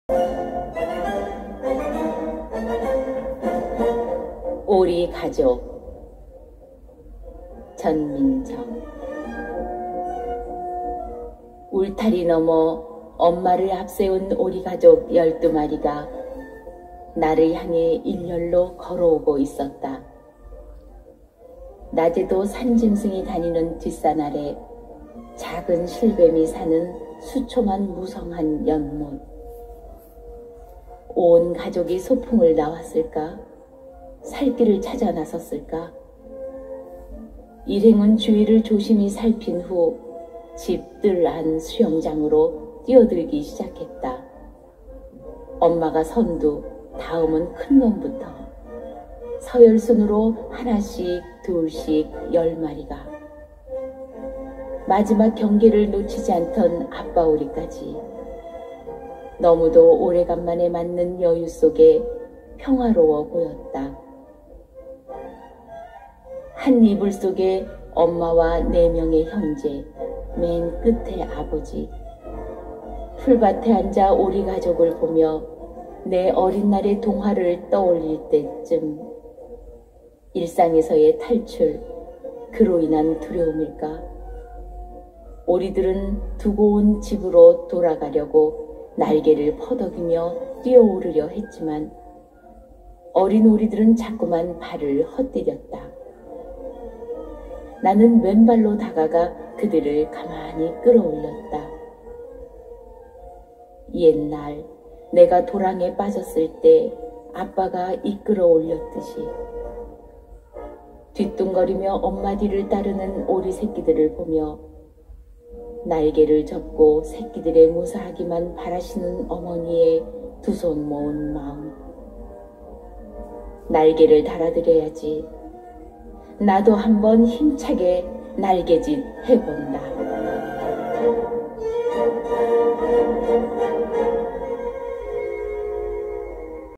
전민정 시인님의 오리가족 함께 합니다.
시인의 맑은 목소리로 시인의 시를 감상 합니다.함께 하심 감사 합니다.🍀🍀